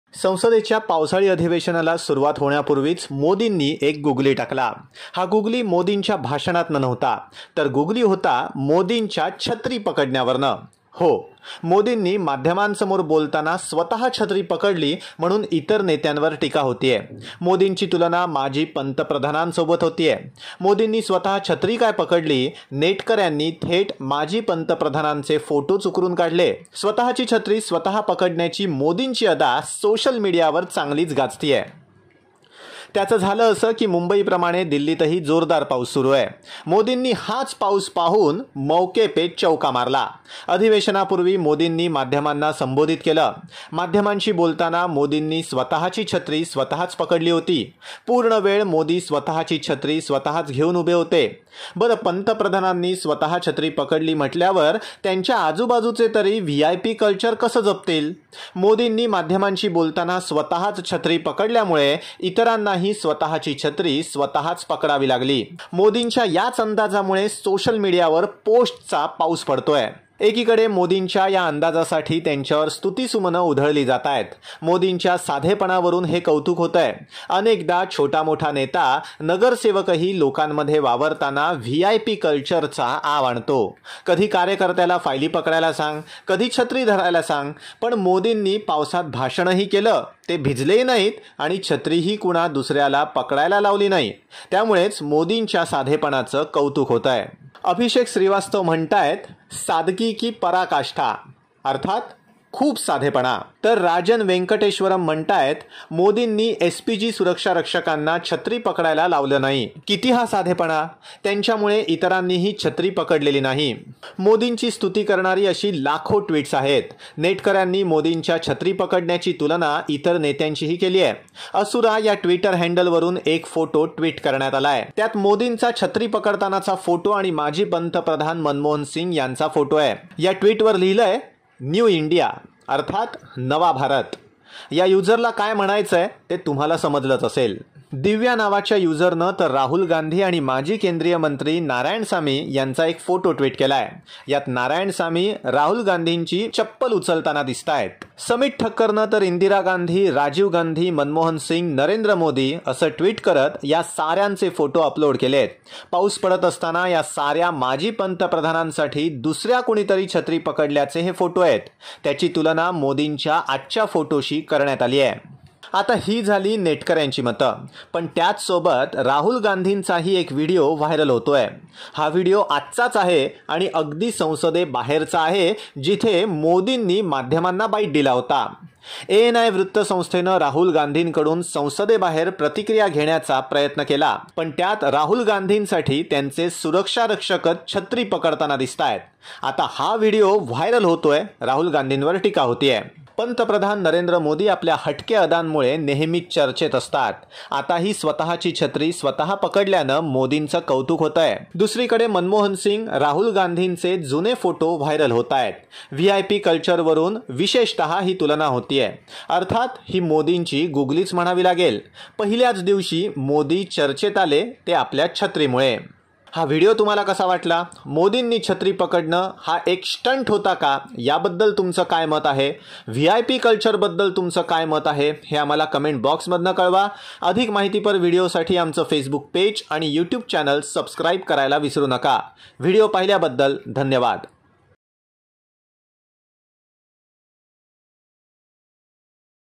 Pm Modi स्वत:ची छत्री स्वत: पकडतात..इतरांचं काय? Monsoon Session 2021 |PM Modi holding his umbrella
संसदेचं पावसाळी अधिवेशनाला सुरुवात होण्यापूर्वीच मोदींनी एक गुगली टाकला. हा गुगली मोदींच्या भाषणातनं नव्हता. तर गुगली होता मोदींच्या छत्री पकडण्यावरनं.. हो.. मोदींनी माध्यमांसमोर बोलताना स्वत छत्री पकडली म्हणून इतर नेत्यांवर टीका होतेय. मोदींची तुलना माजी पंतप्रधानांसोबत होतेय. मोदींनी स्वत छत्री काय पकडली, नेटकऱ्यांनी थेट माजी पंतप्रधानांचे फोटोच उकरुन काढले. स्वतची छत्री स्वत पकडण्याची मोदींची अदा सोशल मीडियावर चांगलीच गाजतेय.